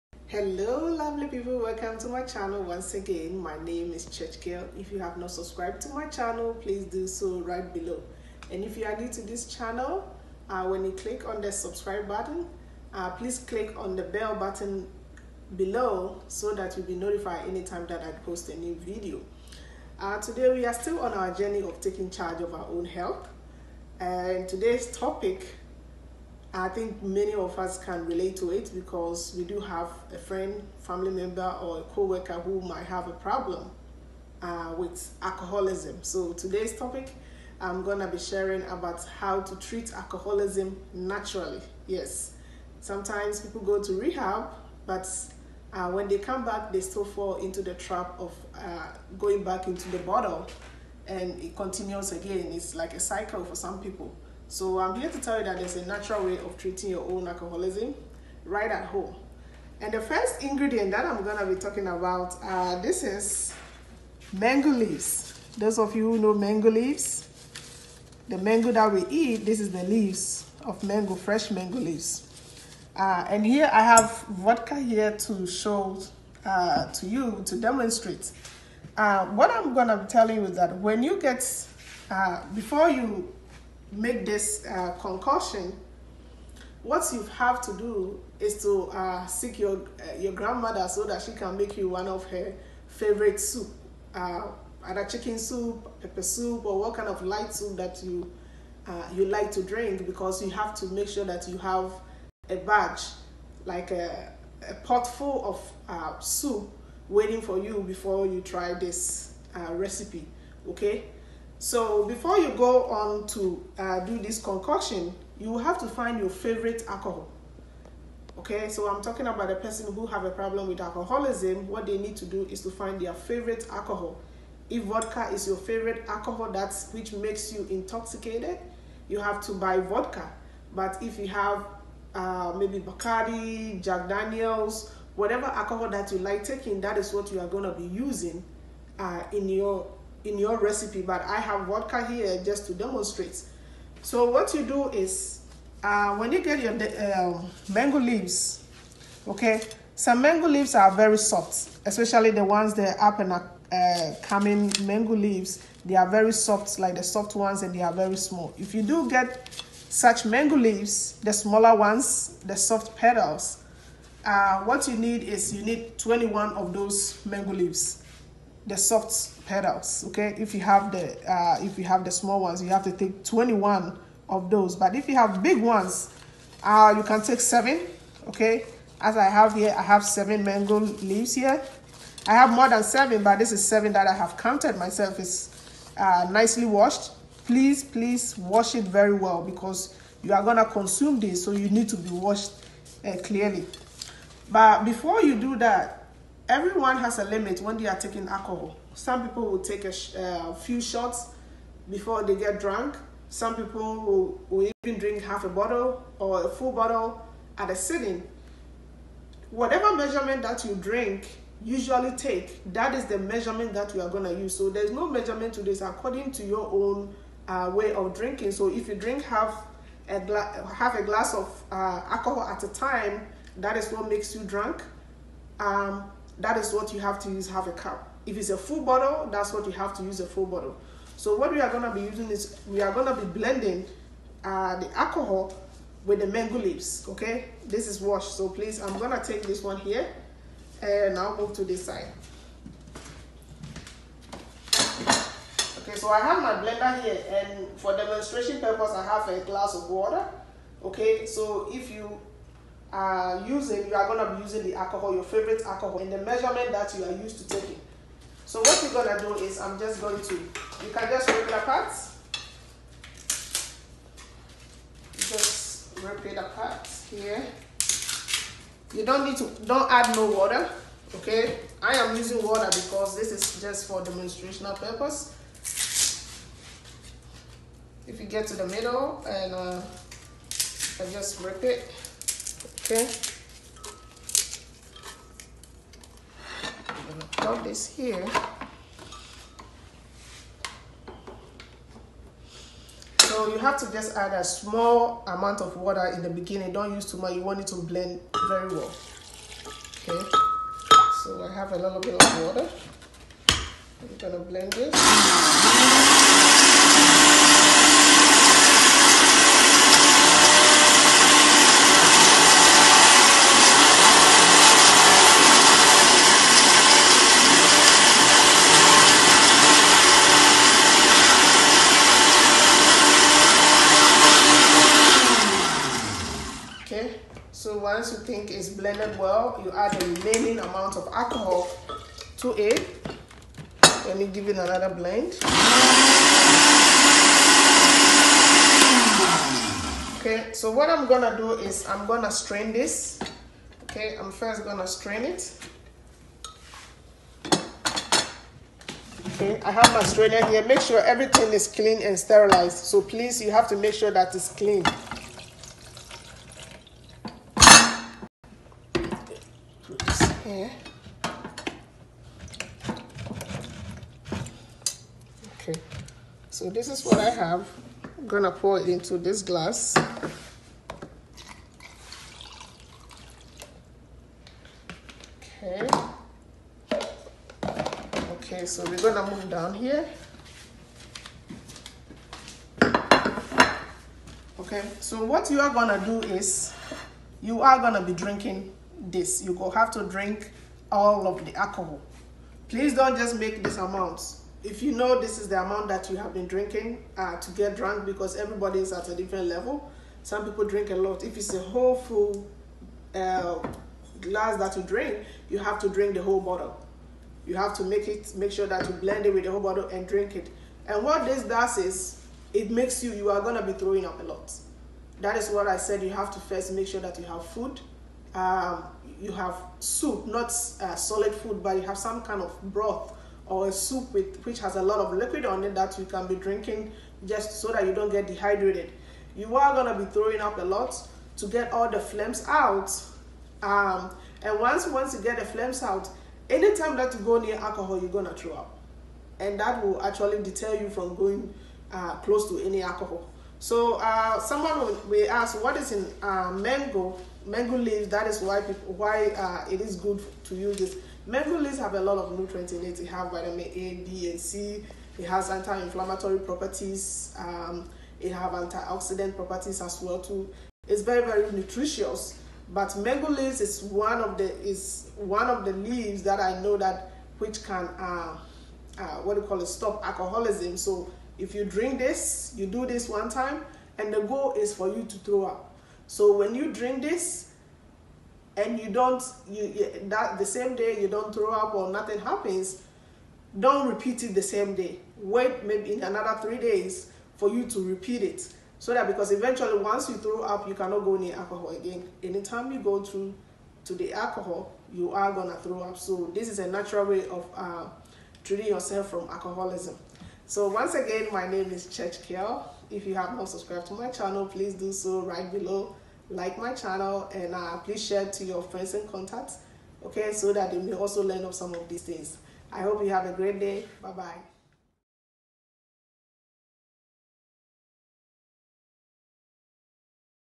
How to Treat Alcoholism Naturally | Home Remedy for Alcoholism.
Alcoholism is the inability to control drinking due both physical and emotional dependency on alcohol. Symptoms include a strong need or urge to use alcohol. Those with alcohol use disorder may have problems controlling their drinking, continue to use alcohol even when it causes problems, or have withdrawal symptoms when they rapidly decrease or stop drinking.

Treatment involves counseling, such as behavioral therapy, and medications that reduce the desire to drink. Some people need medical detoxification to stop drinking safely. Mutual support groups help people stop drinking, manage relapses and cope with necessary lifestyle changes.

Although seeking professional help is the best way to handle alcoholism, there is a NATURAL HOME REMEDY that can stop a person from drinking.

Mango Leaves which has lots of vitamins and many benefits such as anti-inflammatory properties, anti-oxidant properties and many more can WEAN a person from ALCOHOL DEPENDENCE.